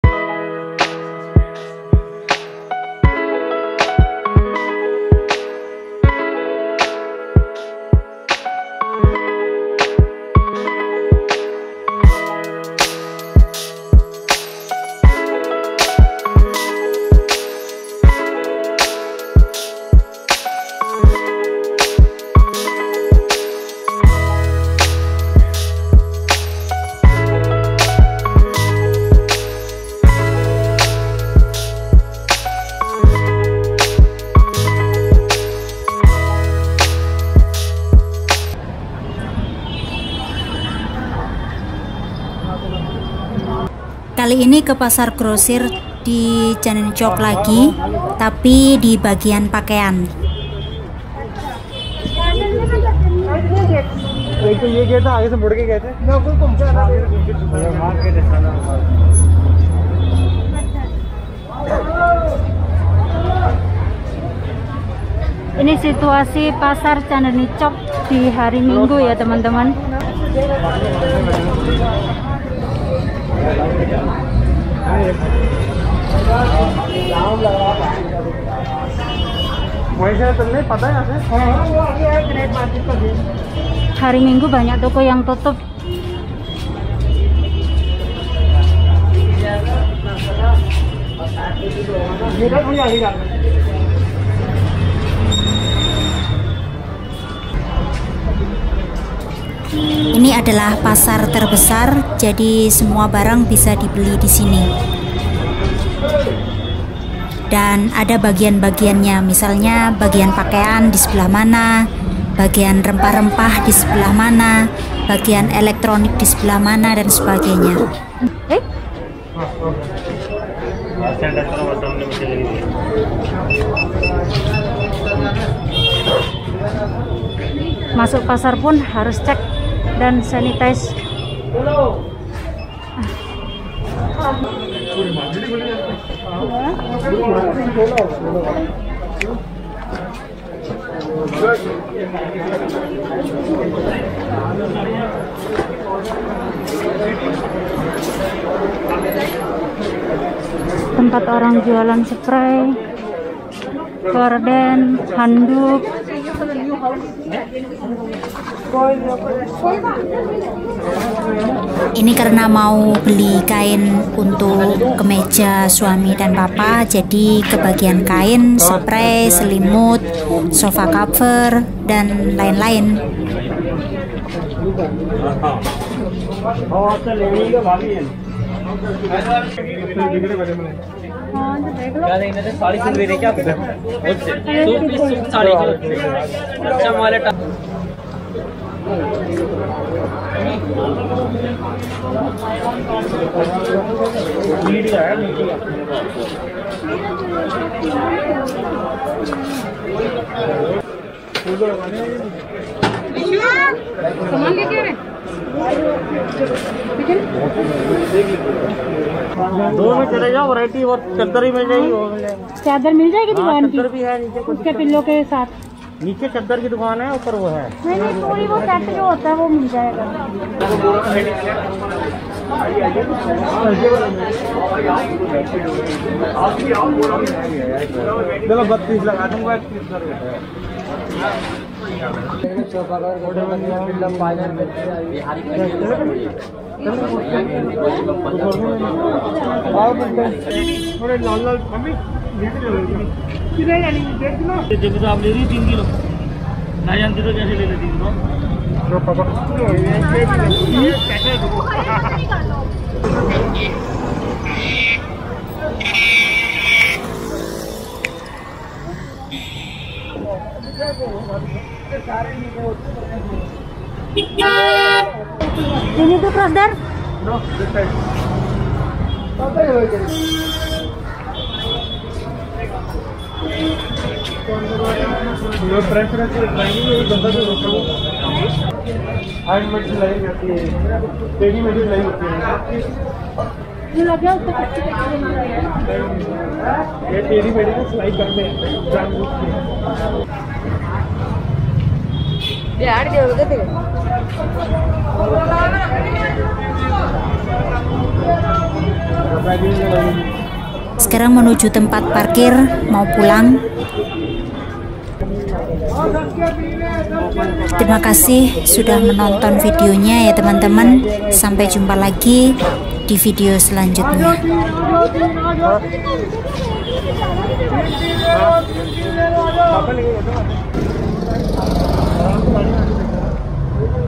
Pasar Grosir Terbesar | Chandni Chowk Market
Hai semuanya.
Kali ini video saya explore pasar lagi yang ada di Chandni Chowk Old Delhi. Yang mana sering di jadikan tempat Syuting film Bollywood.

Seperti apakah pasarnya?
Yuk tonton videonya disini.